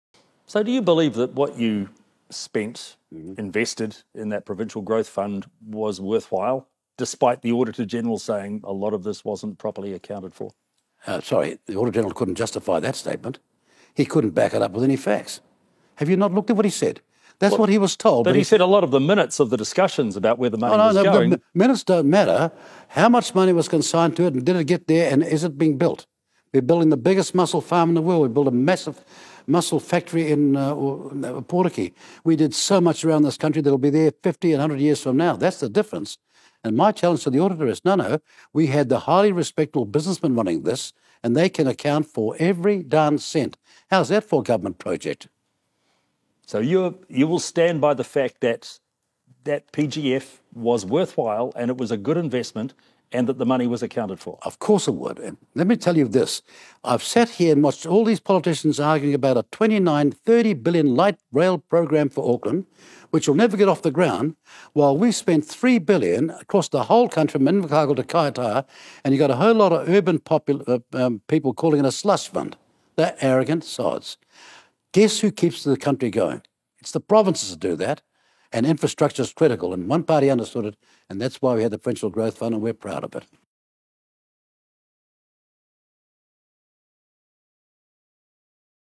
Winston Peters Discusses The Provincial Growth Fund With Peter Williams
New Zealand First leader Winston Peters sat down with Reality Check Radio's Peter Williams to discuss New Zealand First's record on infrastructure and the Provincial Growth Fund.